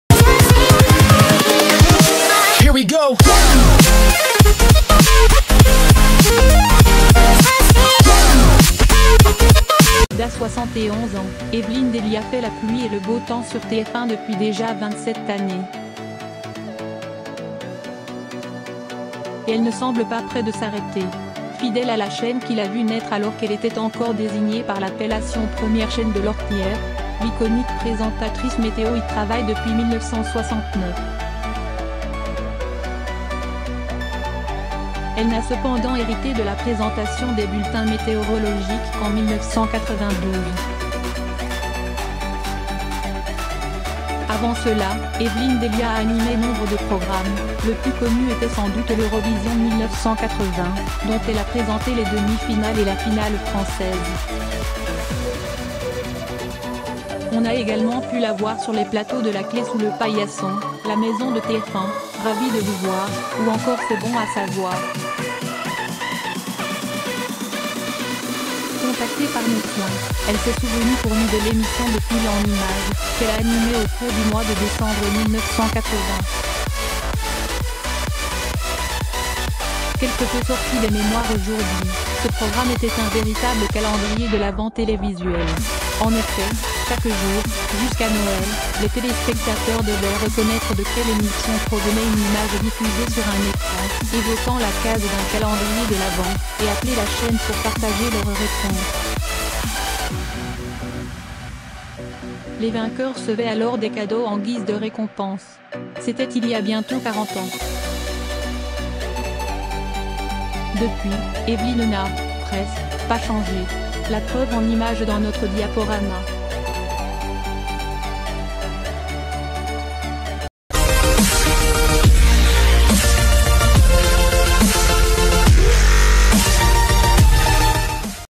Evelyne Dhéliat a 71 ans aujourd'hui : retour sur sa carrière en images
DÀ soixante-et-onze ans, Evelyne Dhéliat fait la pluie et le beau temps sur TF1 depuis déjà vingt-sept années... et elle ne semble pas près de s'arrêter ! Fidèle à la chaîne qui l'a vue naître alors qu'elle était encore désignée par l'appellation "première chaîne de l'ORTF", l'iconique présentatrice...